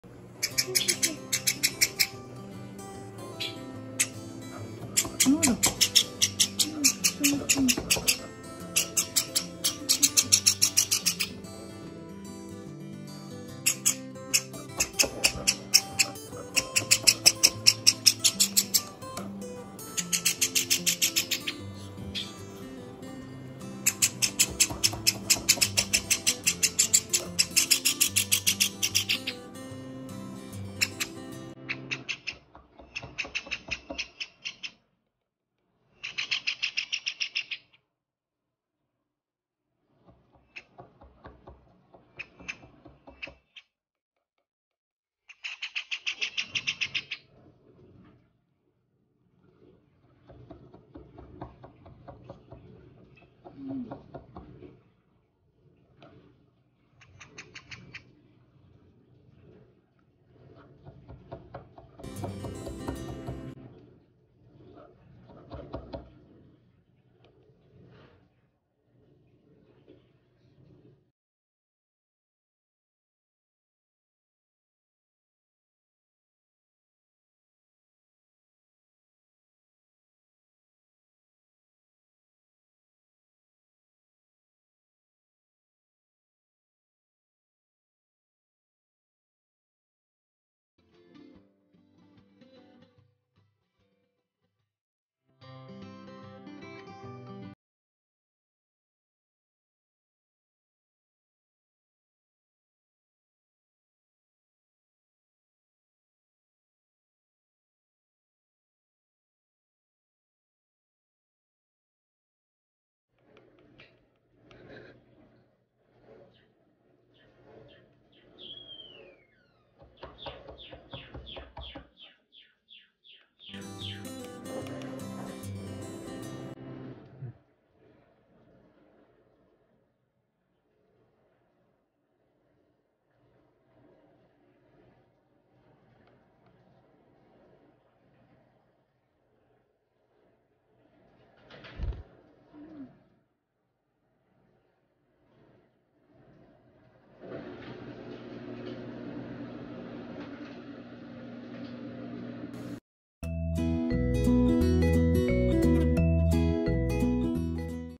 大きな鳴き声でさし餌を要求するひな文鳥こむぎ 癒し動物 面白い動物 Java sparrow
文鳥２羽と暮らしています♪

シルバー文鳥よもぎ（男の子）　2020.1.4〜
シナモン文鳥こむぎ（まだ不明）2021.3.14〜

★LINE公式アカウントを作ってみました！登録お願いします♪★